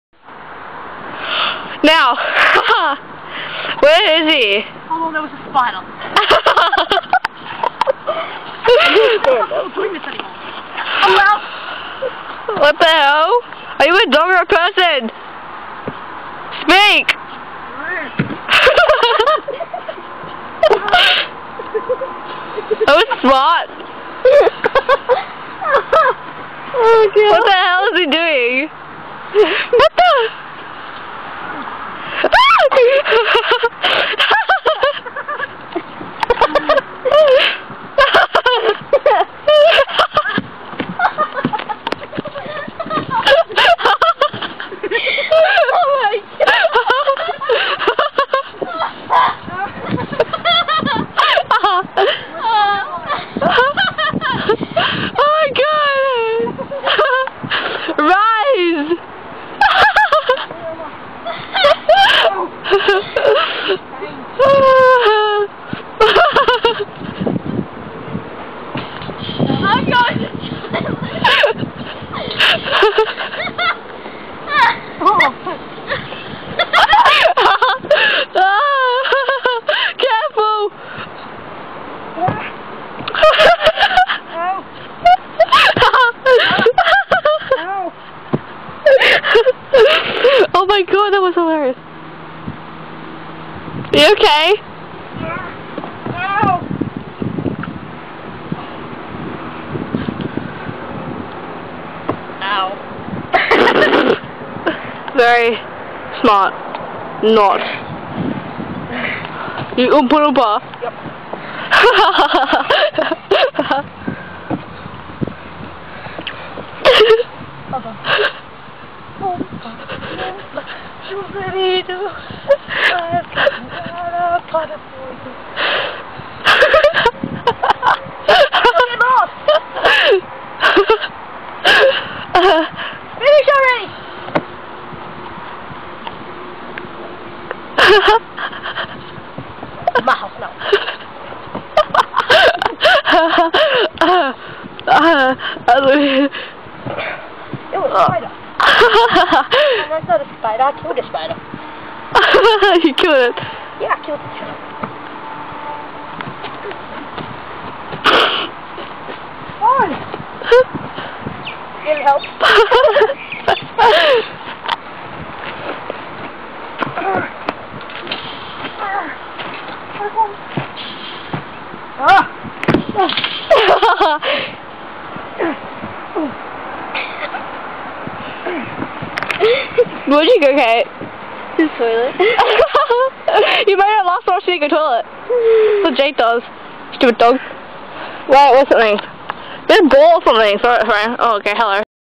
Spider Infested Dog Kennel
Jordy, back for more at my house. This time attempting nothing but getting inside a doghouse, and possibly breaking it, while NOT getting bit by spiders.